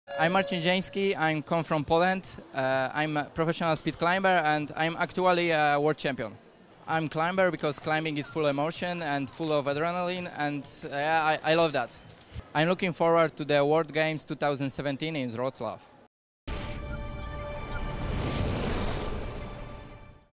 Marzin Dzienski - Speedclimbing World Champion
Marzin Dzienski is looking forward to the World Games 2017.